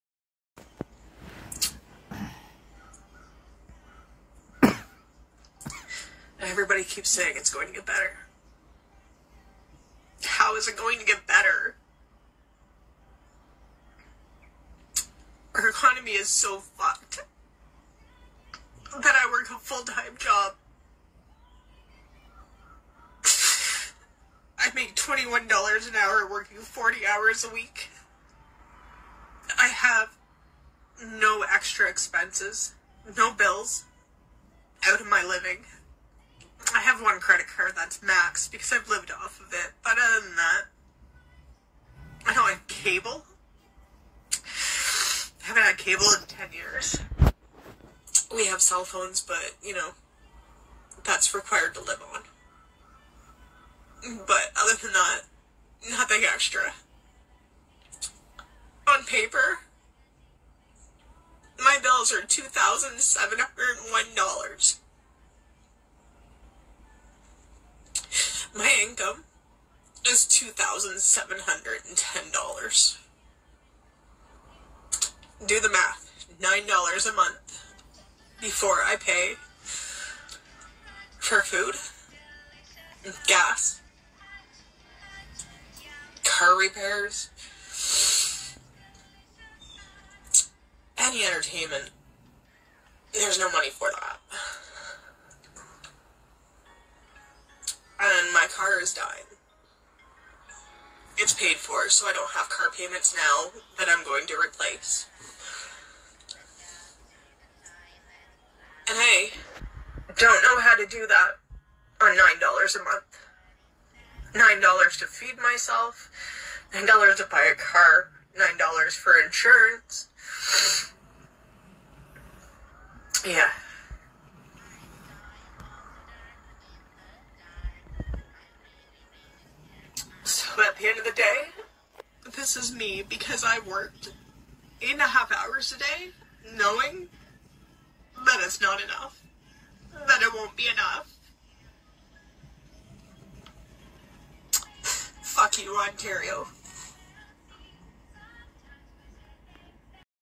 A Woman Expresses How High Cost Of Living In Canada Impacts Her Life
Watch a woman expresses how she is doing with high Cost Of Living crisis. Watch it full and let me know what you think about it